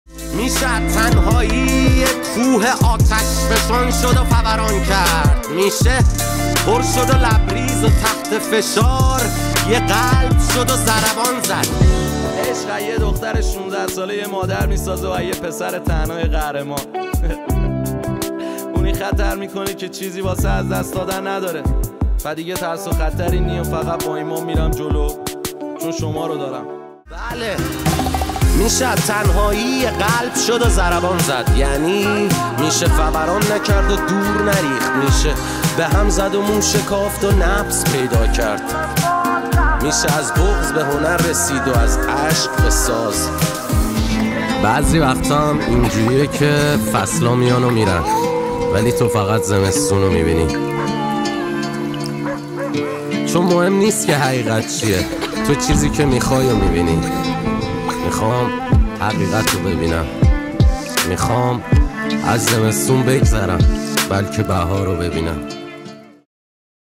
امضای هواداری، هدیه امیر تتلو
به خاطر شرایط امنیتی و سواستفاده از امضای قبلی امیر تتلو، او این امضای هواداری رو درست کرده که هواداراشون این امضا رو یادگاری داشته باشند و این دست نوشته و امضای جدید امیر تتلو هدیه امیر تتلو برای اشخاصی که درخواست امضا کرده اند…
(ادمین)